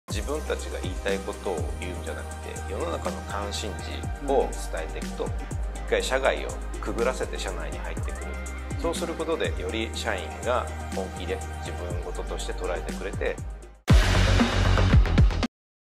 【ユニークなCMの制作秘話も】サイボウズの企業ブランディング
【フルverはこちら】

【そのまま提案資料に使えるプレゼンスライドはこちら】

【この動画のイベントレポートはこちら】

【概要】
SNSが台頭し、Web上には情報があふれかえる「情報過剰時代」。従来に比べるとブランディングの手法も多様化し、自社の存在価値をユーザーや社会にどう発信すればいいのか、悩める経営者、広報・PR責任者の方は多いはずです。

そこで今回は、「社会浸透する企業ブランディング」をテーマにサイボウズでコーポレートブランディング部長を務めている大槻氏をお招きしました。

企業ブランディングの成功事例として著名なサイボウズが、果たしてどのように現在のような確固たるブランドを確立していったのか。大槻氏自身のご経験を基に、たっぷりお話しいただきました。